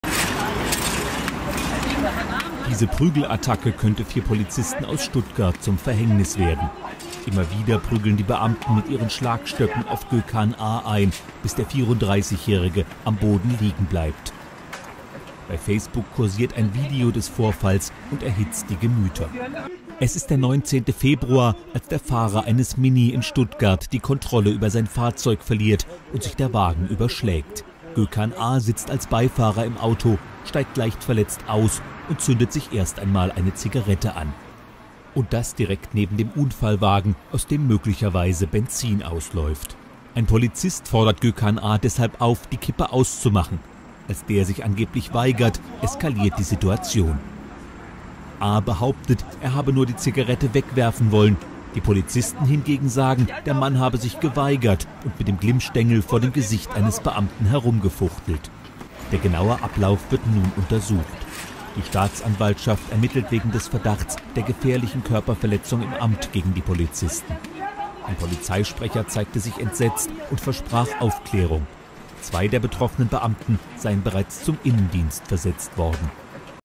Türke wird von rassistischer deutscher Polizei brutal zusammengeschlagen. Menschenrechte in DE!
Rassismus in der Polizei gibt es nicht nur in den USA. Wie im Fall Rodney King wird auch in Deutschland Gökhan, Angehöriger der türkischen Minderheitsgesellschaft, brutal von der deutschen Polizei zusammengeschlagen. Wer ernennt solche Monster zu Polizisten, die doch den Bürger eigentlich schützen sollen? Warum sind Minderheitsangehörige immer wieder Opfer deutsche Staatsgewalt? Warum wird das immer wieder versucht zu vertuschen. Warum gibt es keine Minderheitsangehörige in oberen Positionen der deutschen Polizei, die mitgestalten und solche Gewaltexzesse der deutschen Polizei besser verhindern und Akzeptanz in der Minderheitsgesellschaft schaffen könnten?